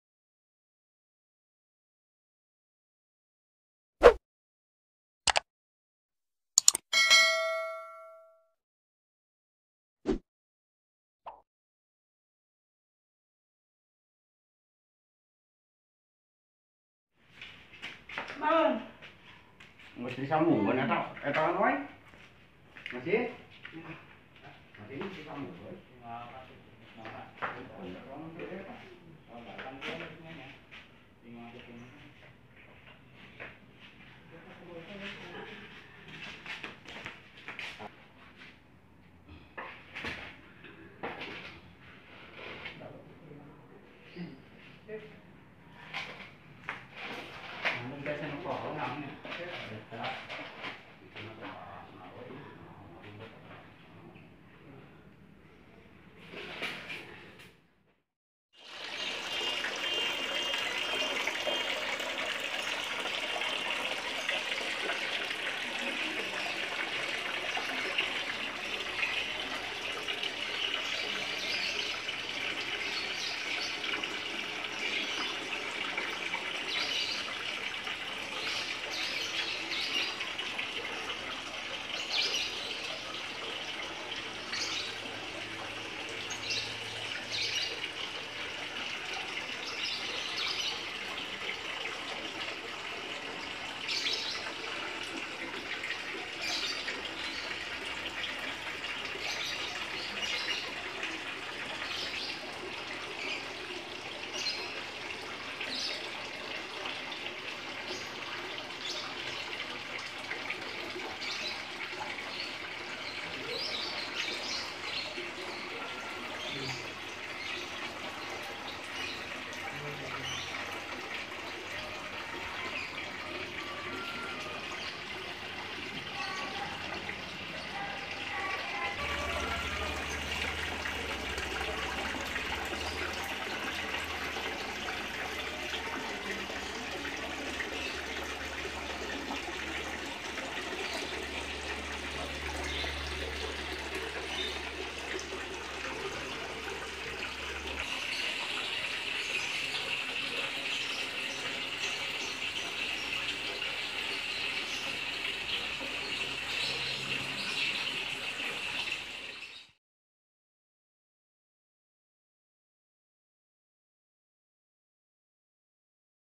Taman sederhana dan kolam di lahan sempit
Bikin taman dan kolam di halaman rumah dengan lahan yang sempit